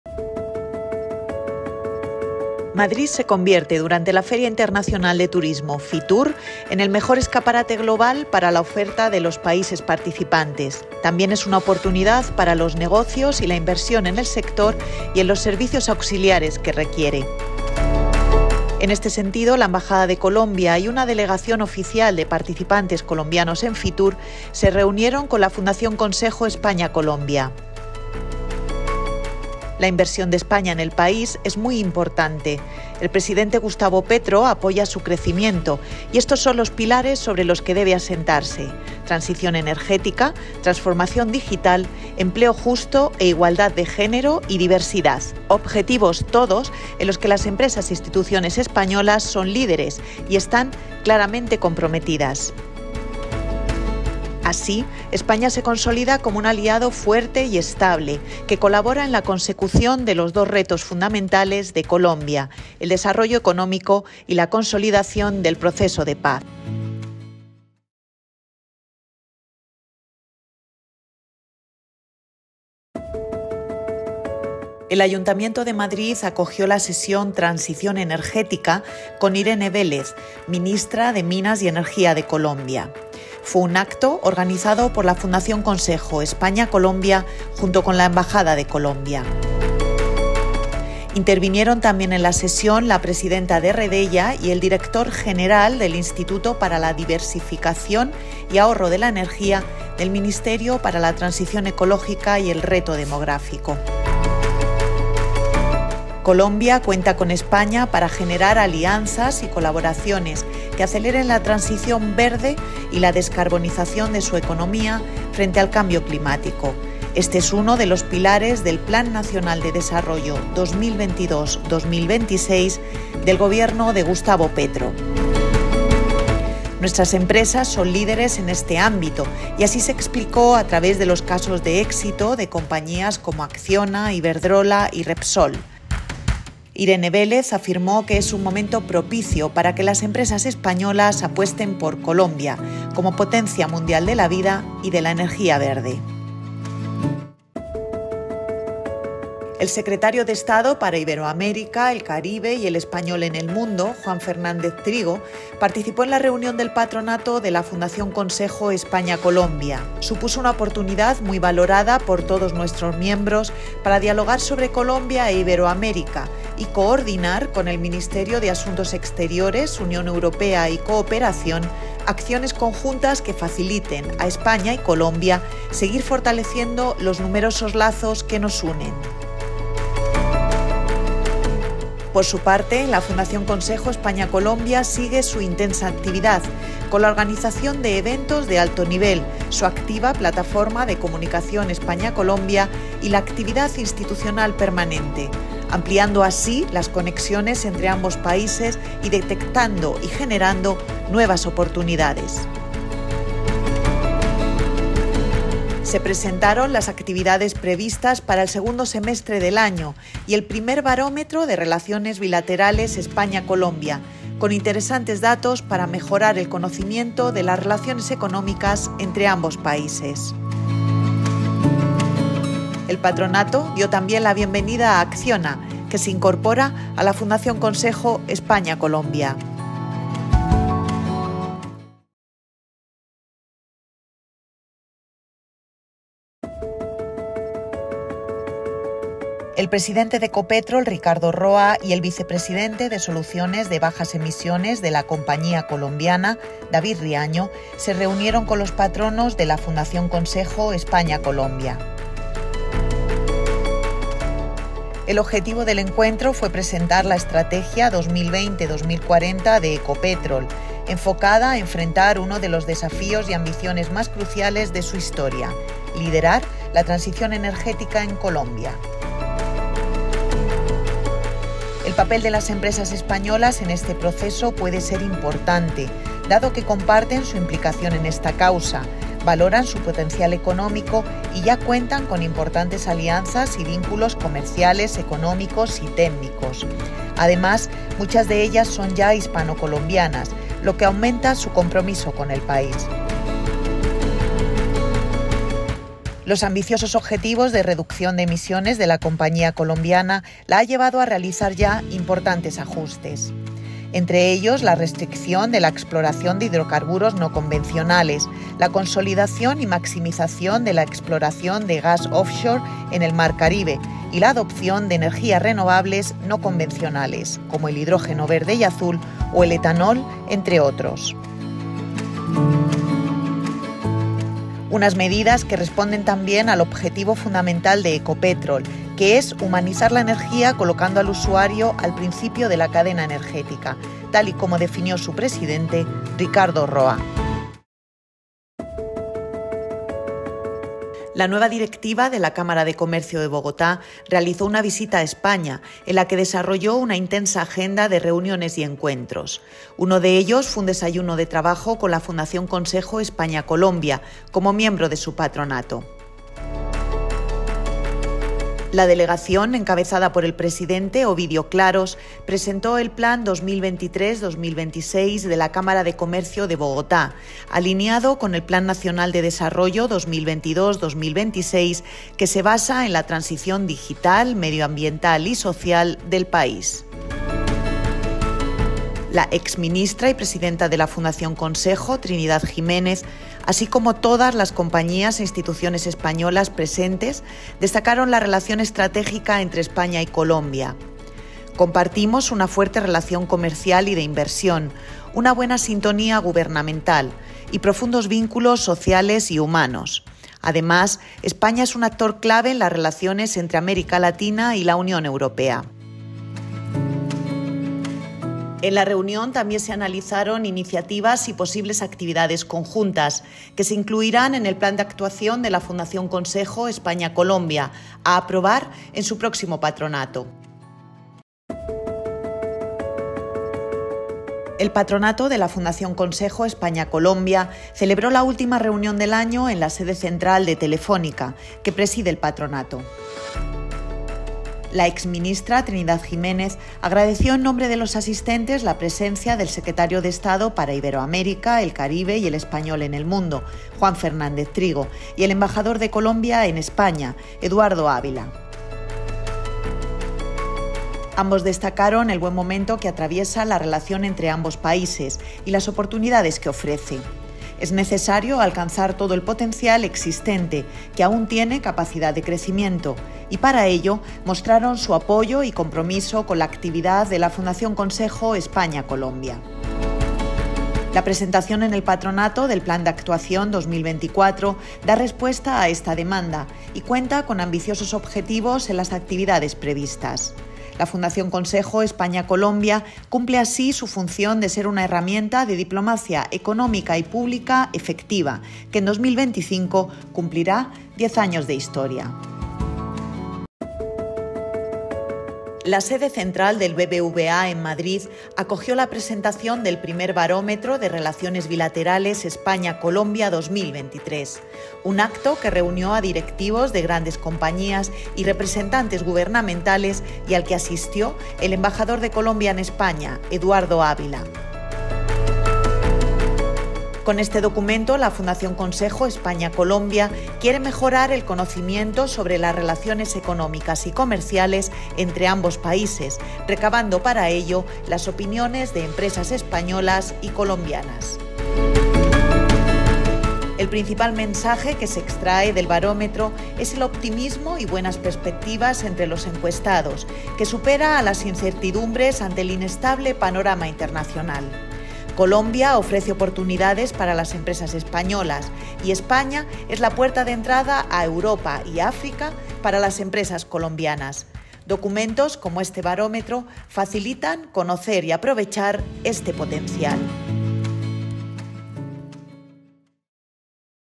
Resumen anual audiovisual de la Fundación Consejo España-Colombia en 2023
Terminamos un año intenso, con un amplio número de visitas institucionales y del sector privado entre nuestras actividades. También

Este vídeo resume la actividad de la Fundación Consejo España-Colombia. Entre nuestros actos más destacados:

· Presidente de Ecopetrol se reúne con la Fundación Consejo España-Colombia
· Presentación del I Barómetro de relaciones bilaterales España-Colombia 2023
· Reunión con la nueva directiva de la Cámara de Comercio de Bogotá
· Seminario sobre Transición Energética con la ministra de Minas y Energía
· Reunión con la delegación participante en Fitur